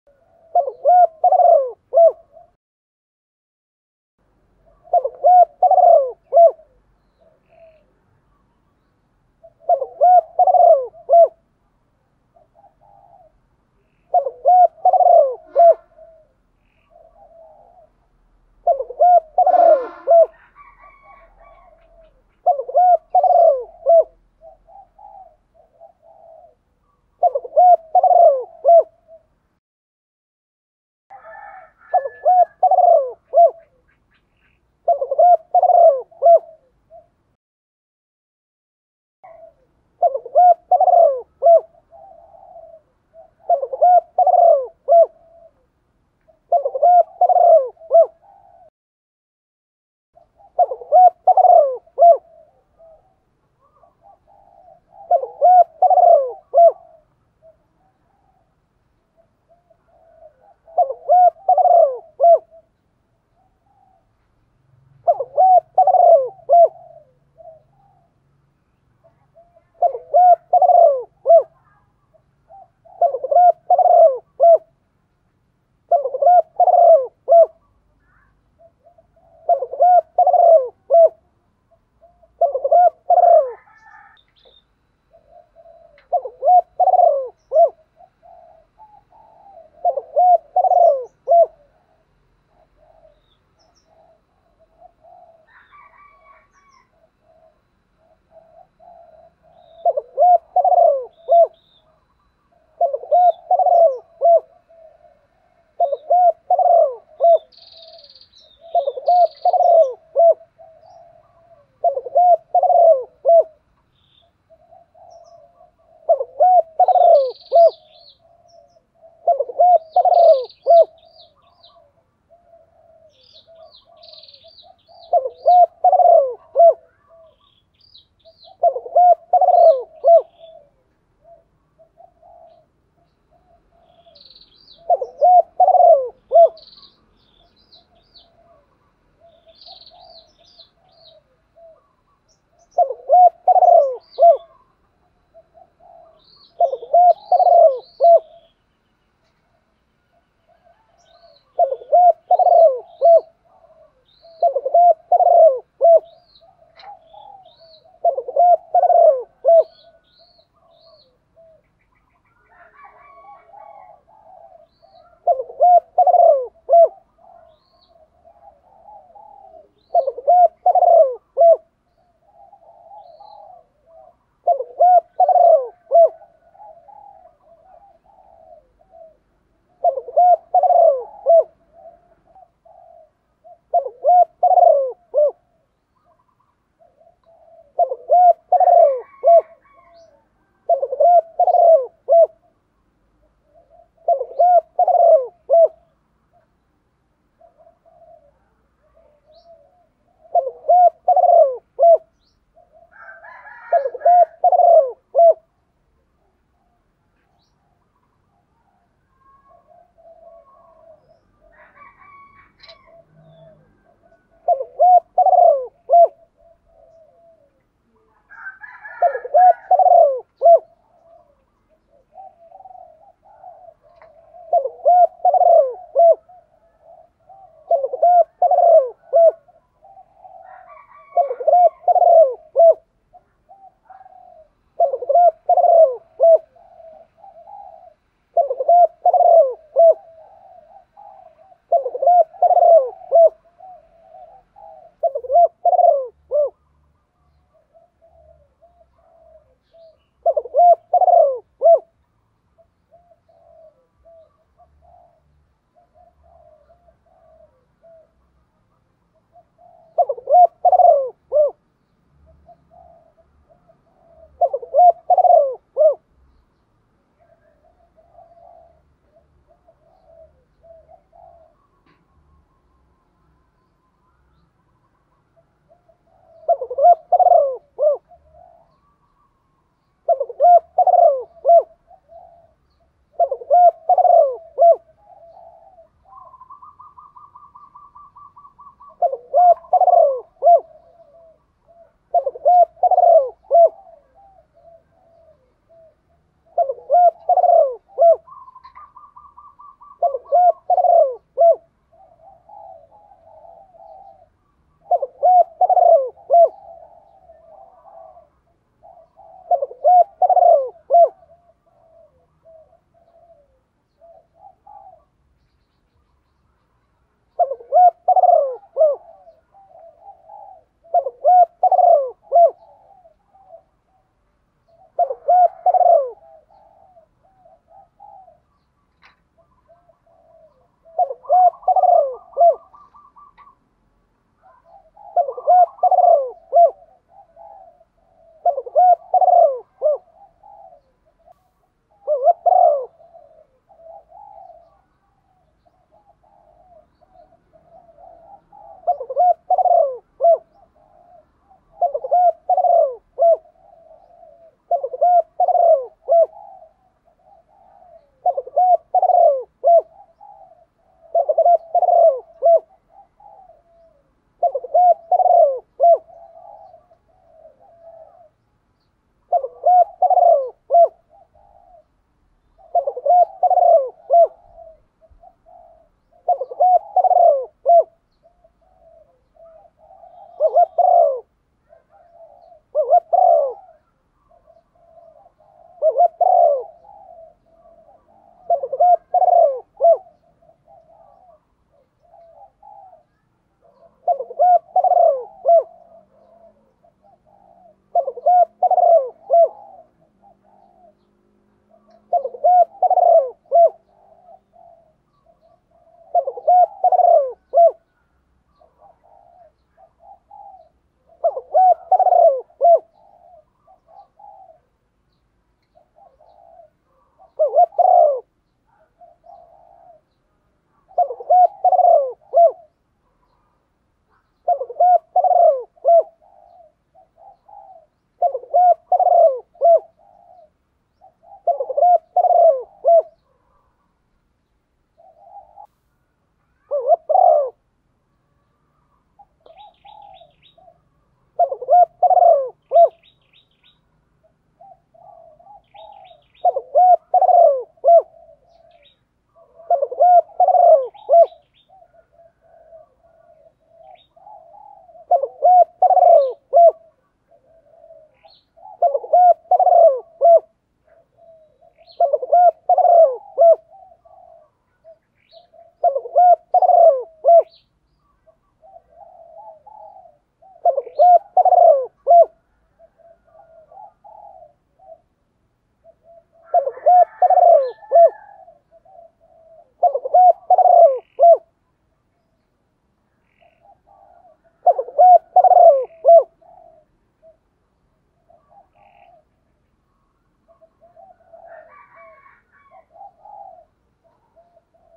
cu gáy giọng thổ pha thúc đấu dút chim bổi nhanh gáy, và về cội nhanh hơn
cu gáy giọng thổ bầu chuẩn nhất hiện nay